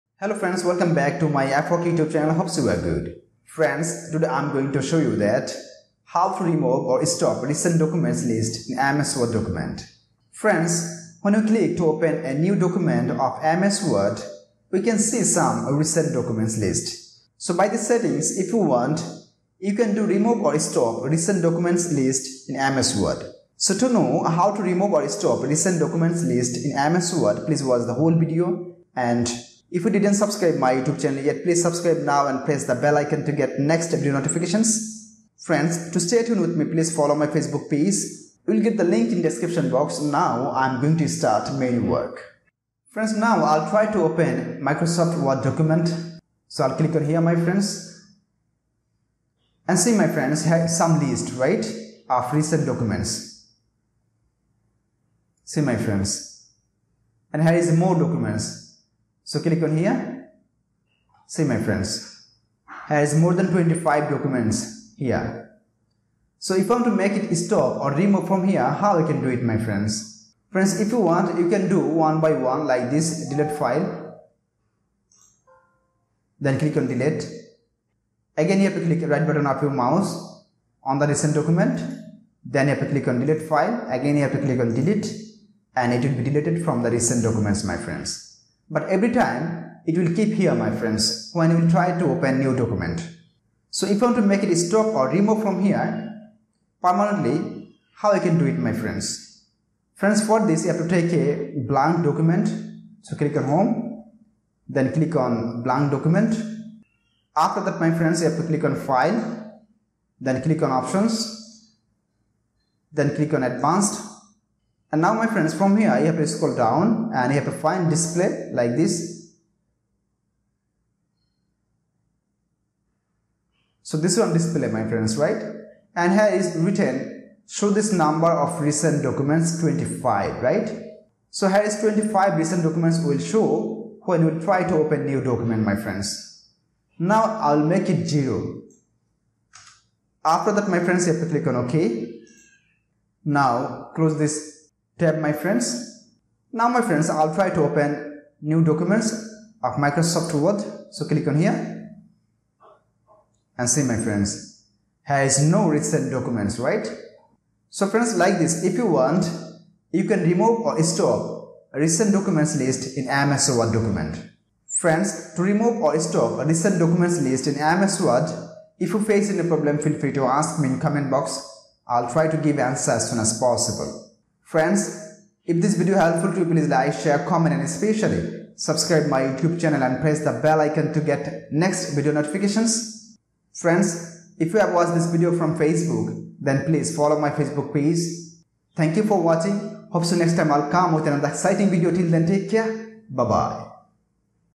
How to Remove Recent Documents List in MS Word | How to Stop Recent Documents List in MS Word
Hello Friends,
In this video you will find that how to remove or stop recent documents list in ms word. Friends when we click to open a new document of ms word, we can see some recent document list. So by these settings if you want you can do remove/stop recent document list in ms word. So to know how to remove recent document list from ms word or how to stop recent document list from ms word please watch the whole video and if this video helpful to you please like,share,comment below.

From
F HOQUE.